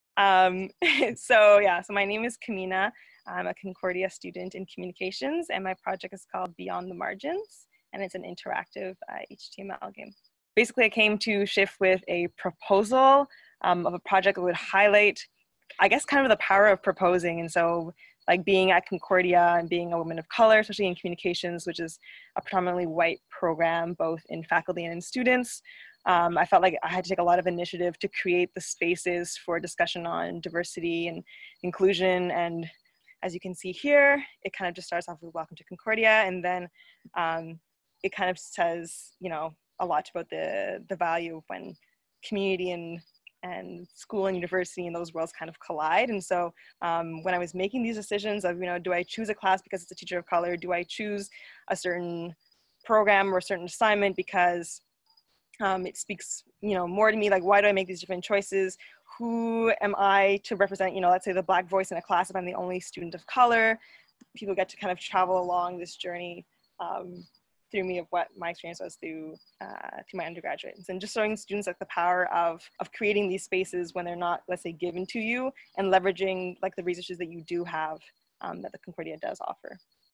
Spark! Campaign Celebration: Camina Harrison-Chery
The culmination of 3+ years of consultative work with students, faculty members, and student life professionals at Concordia. To celebrate, we have selected 8 student artists to exhibit how and why they engage on campus. The exhibition of their work, and the facilitated discussion in relation to their creative work is entitled Depictions of Engagement.